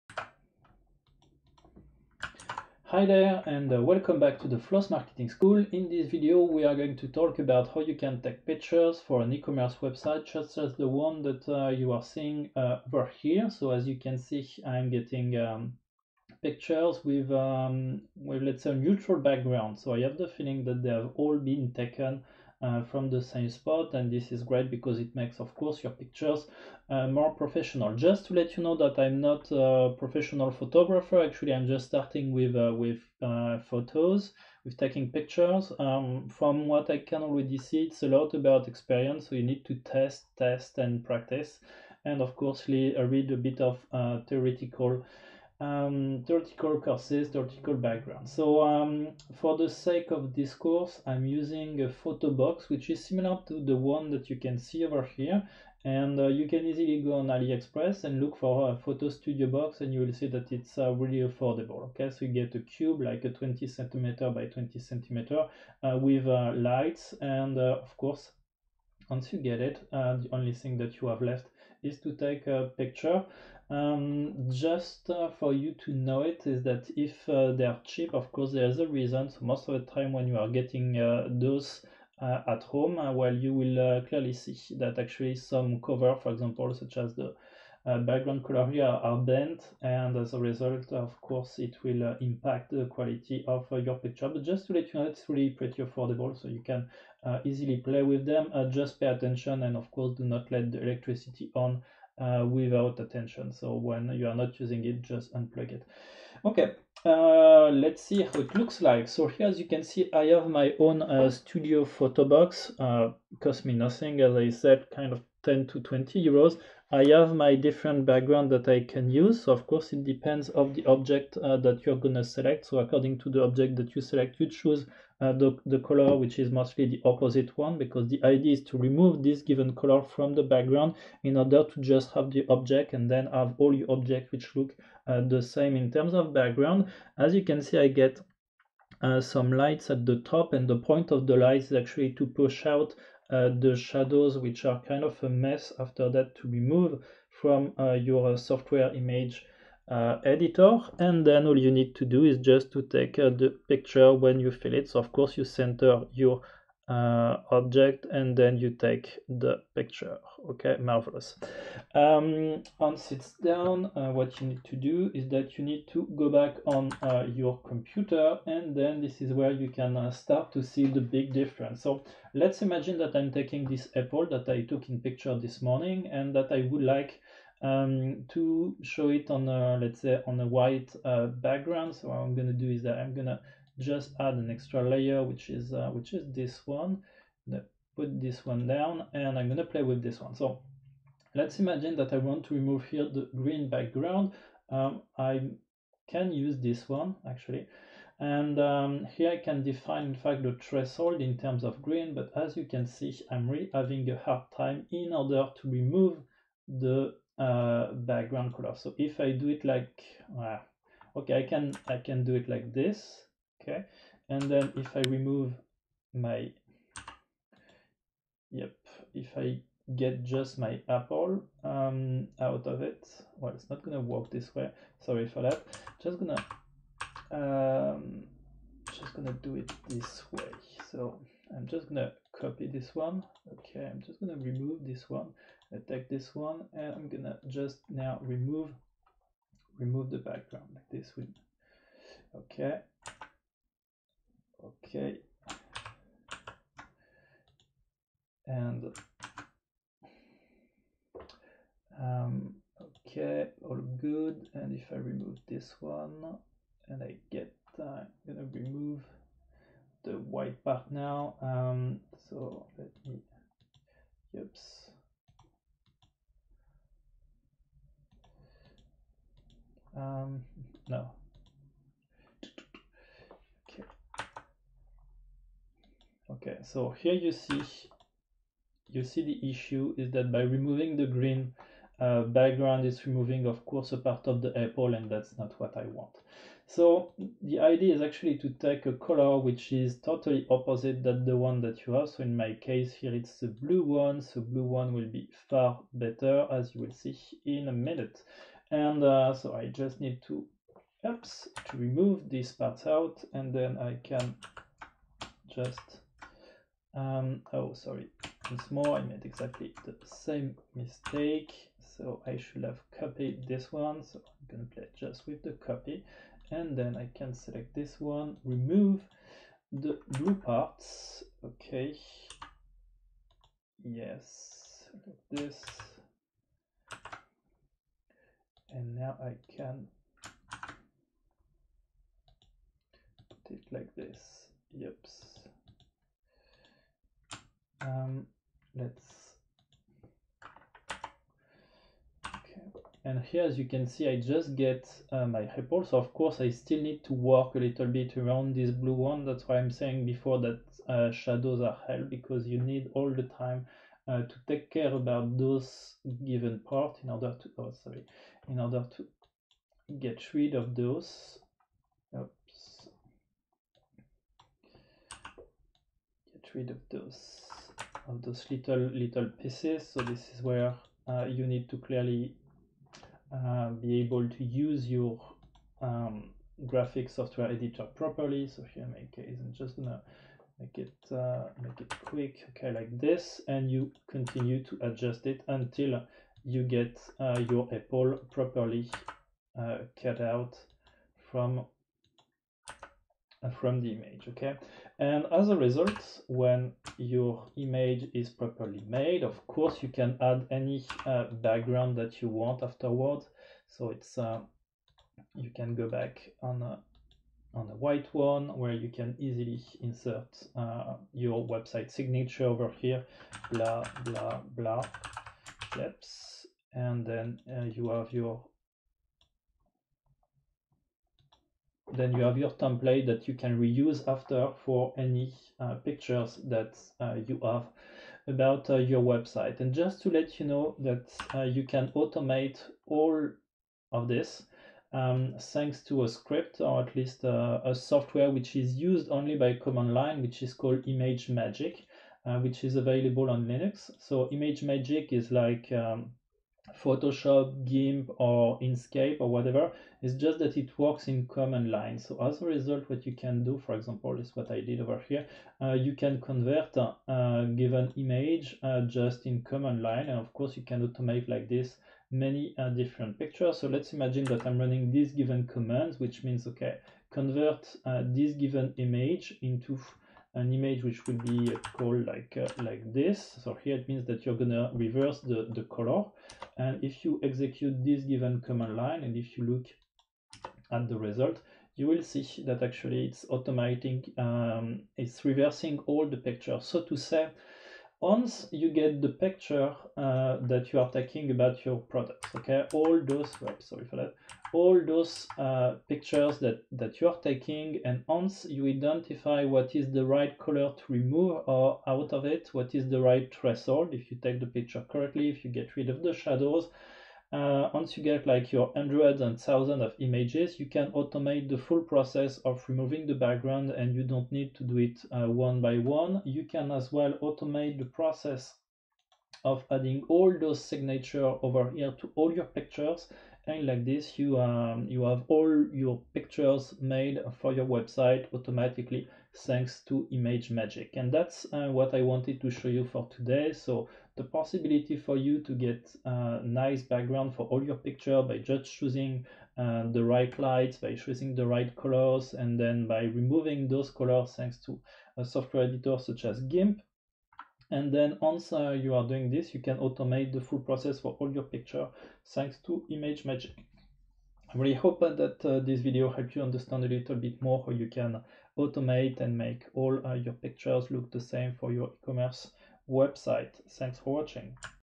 Taking product pictures for your website
How can you take great pictures for your website if you are not a professional? Well, you need to become one... and to become one you need to try. In this video I am showing you how to get started.